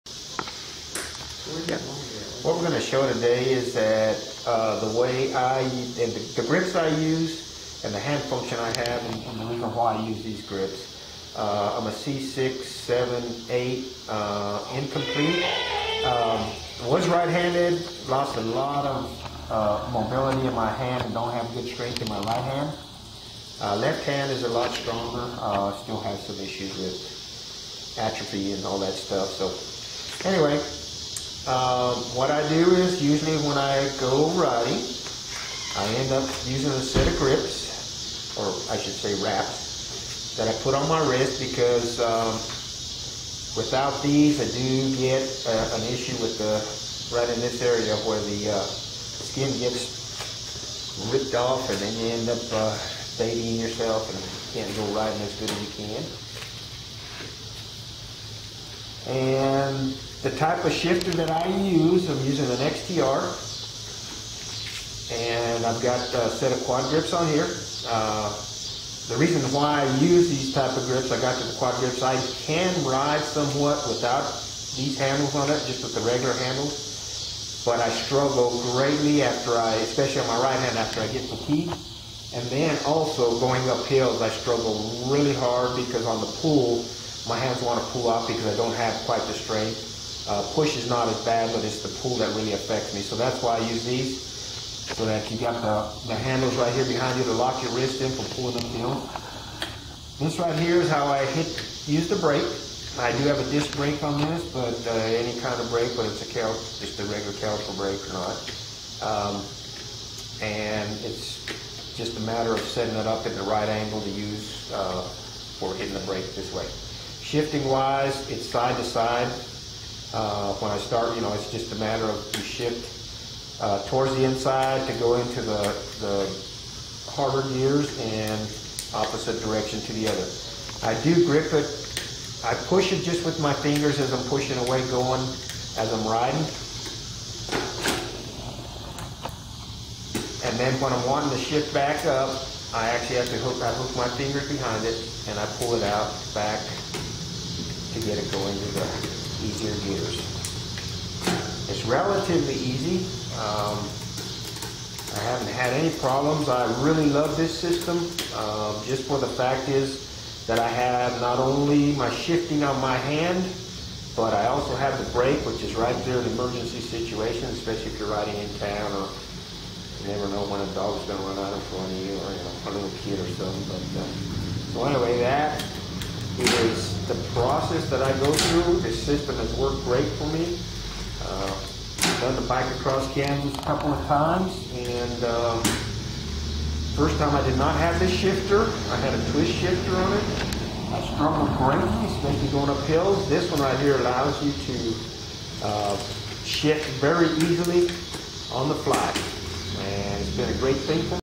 C6/7/8 incomplete Quad, some hand use- Shift & brake on 09 R with QuadGrips from QuadGrips.com.
Wayne Romero using 2011 QuadGrips(functionally same as newest generation) and XTR (same as XT ) single lever shift & Brake. Wayne is a C-6/7/8 incomplete quad with fairly decent function in his hands. Left hand is best. This method only works for quads that have good return in at least one hand.